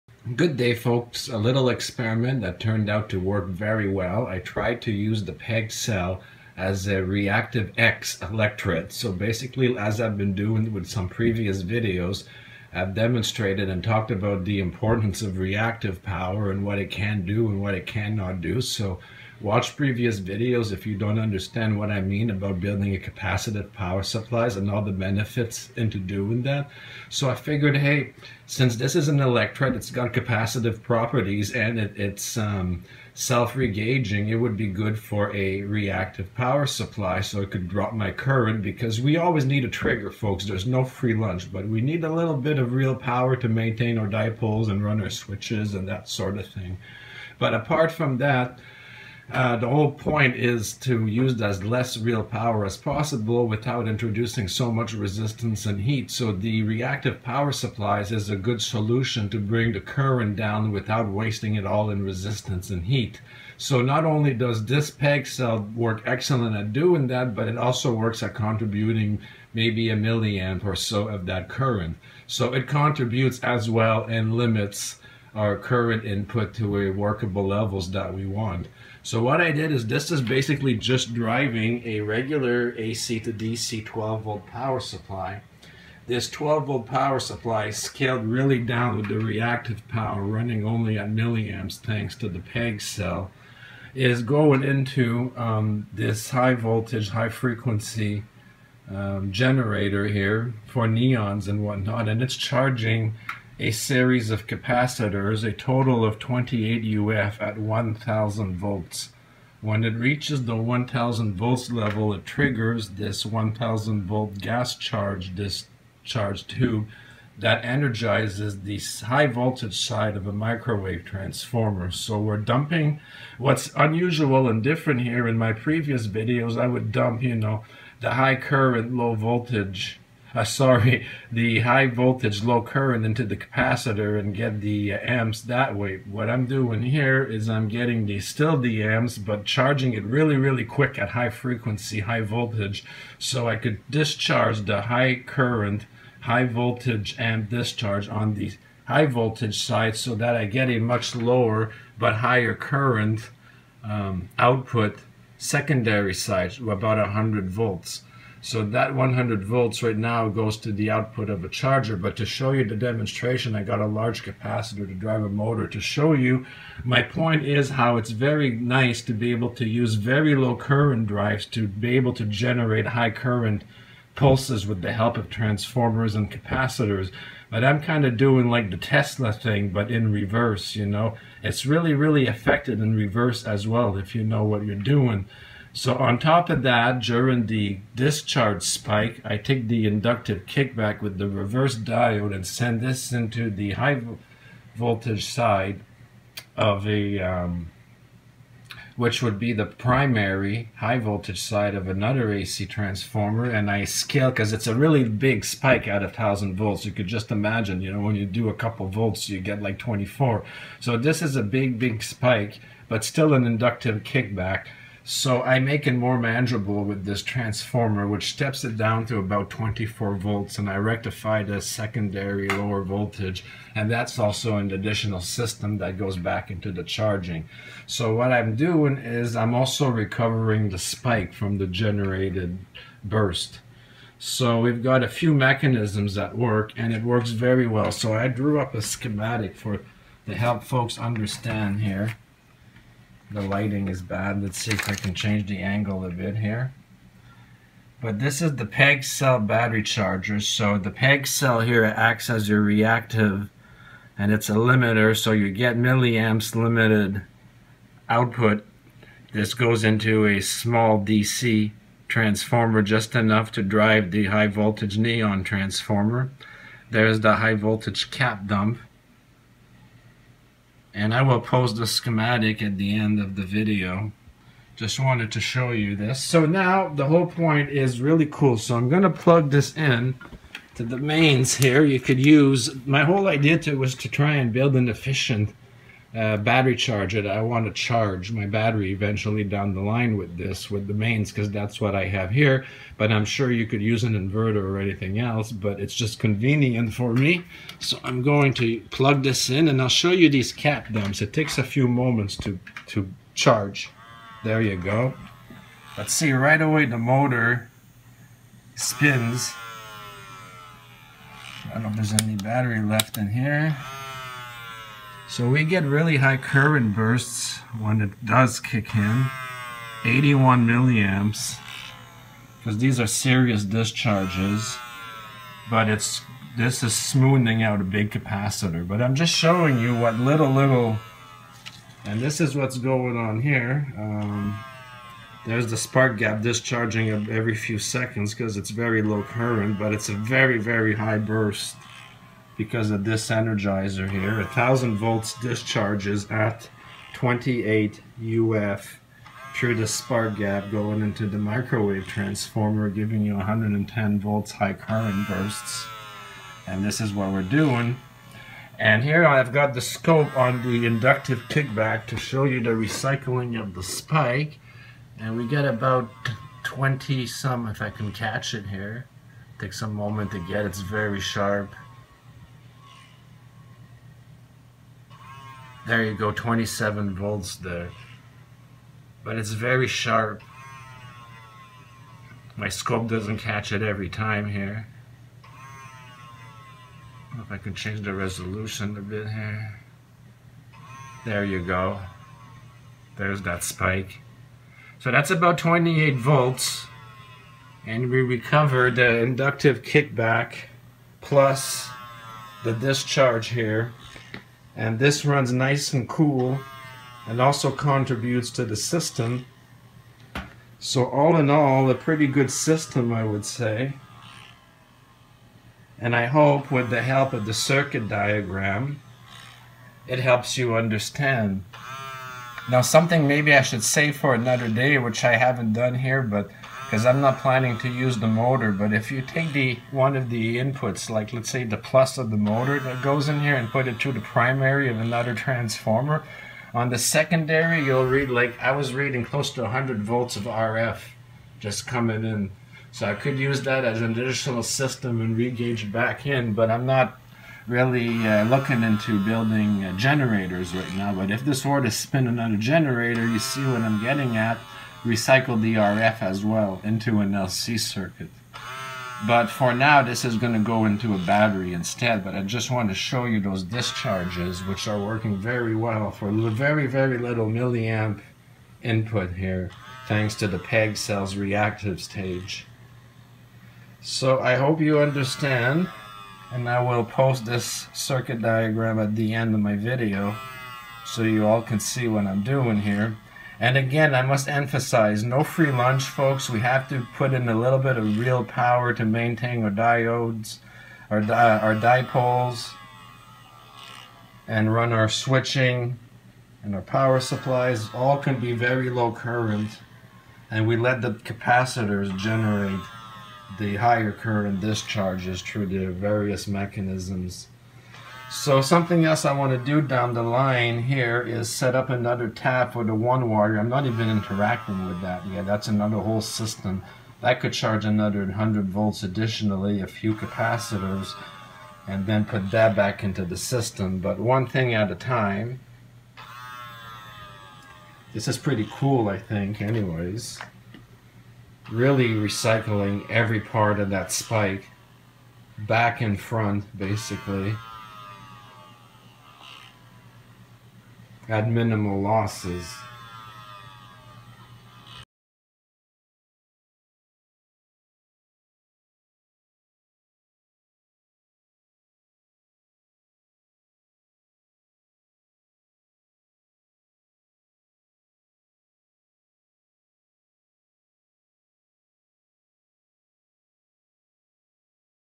PEG Cell Battery Charger
Good day, folks! Today, I’m excited to show you how to turn the PEG cell into an efficient, and potentially over-unity, battery charger. The efficiency and performance largely depend on how you implement and tune the system.Here’s what I’ve discovered: the PEG cell can act as an X reactive electret, similar to a normal reactive power supply but with added benefits. It operates with reduced current, allowing us to use only milliamp-level trigger power to maintain open dipoles and run our small switching circuits. While it's not a free lunch, we can significantly increase efficiencies and improve couplings with the environment. The key is to keep our dipole open as long as possible, using minimal asymmetric triggers.

Here’s a brief overview of how the system works:Reactive Power Supply: The PEG cell drives a small 12V power supply that powers a high-frequency, high-voltage AC driver.

Capacitor Charging: This driver quickly charges low-current 28μF capacitors to 1kV, limited by the spark gap discharging at 1kV. This acts as a capacitor dump into the high-voltage side of a microwave transformer.
Energy Recycling: The stepped-down higher current, lower 100V pulse is sent to the battery alongside the recovery EMF pulse transduced by the second transformer during the spike. This allows us to recycle many parts of the spike.

Additionally, I take advantage of using multiple capacitors, not just for high voltage rating but for their “memory rebound” effect. The more varied capacitors you have in the series, the more pronounced this effect becomes, leading to more "free energy" during each discharge cycle. This is another way to tap into the efficiency and anomalies of capacitors to our benefit. The discharge rate slowly ramps up.

I can further fine-tune the reactive stage to potentially save a few more milliamps from the already low current active power requirement. By integrating a variac in line with the PEG cell, I can adjust the cell to resonate more effectively with the mains supply. This tuning would enhance the overall efficiency of the system.

In the future, I plan to upgrade this circuit to incorporate a one-wire system and an AV diode plug. I aim to utilize a couple of AV taps on the high-voltage side to tap into the Poynting vector field (measured in watts per square meter). By adding a small antenna for ground-side reflection, we can charge a few small capacitors at 100V and periodically discharge them. This will allow us to harness energy from fields that are typically wasted.For more details, please refer to the circuit diagram at the end of the video. Sorry for taking a while. This circuit took some days the build and test.

Please be mindful that 28 uf 1KV capacitor discharges can be potentially lethal(The heart of our current generation) even if charged at zero/low/electrostatic current. So be very safe and cautious if you are planning to experiment with this design. The capacitor doesn't care.